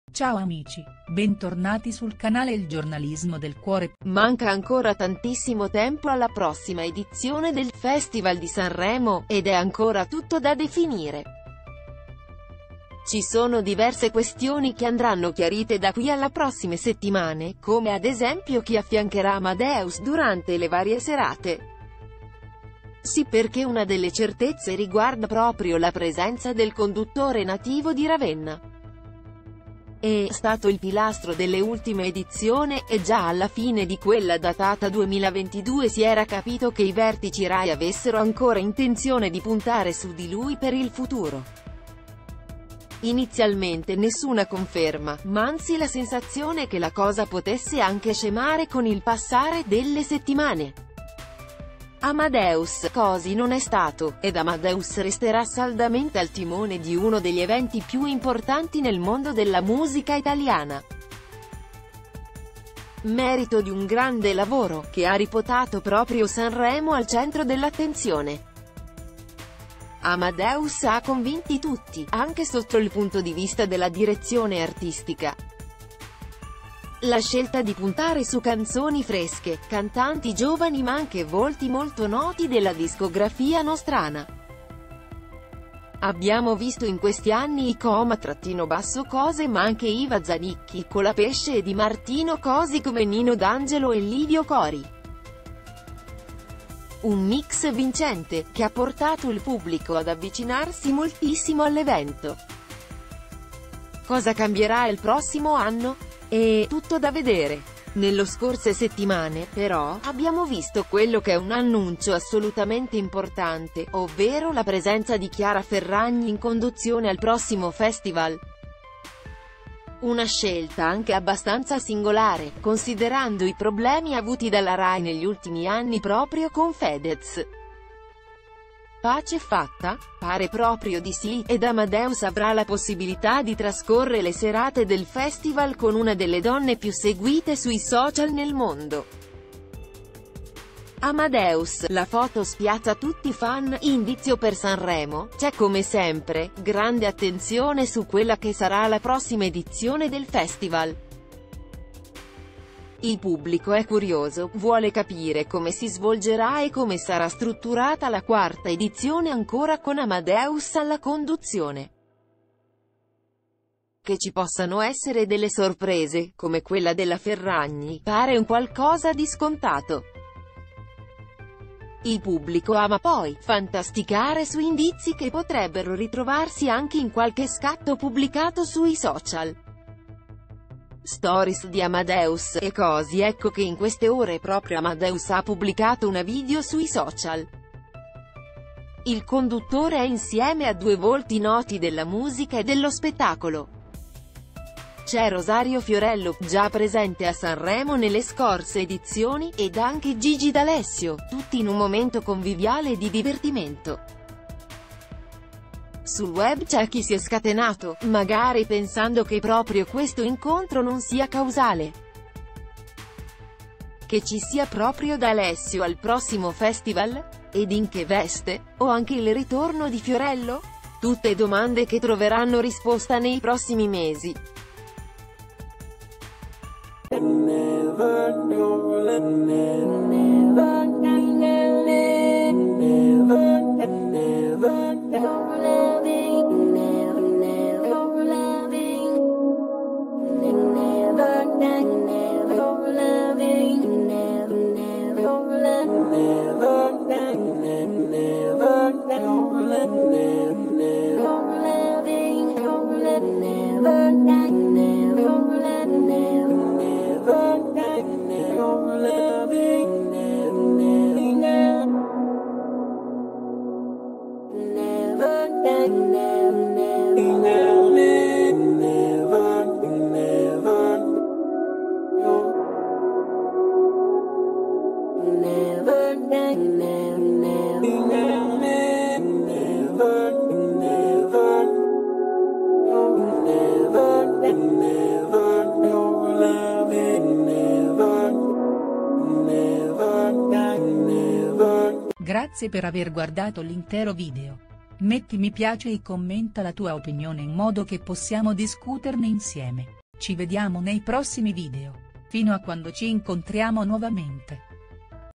Amadeus, la foto spiazza i fan La rivelazione, ci sarà anche lui a Sanremo
Il video sui social ha mandato in tilt i fan. Un indizio per la prossima edizione del ‘Festival di Sanremo’, ci sarà anche lui?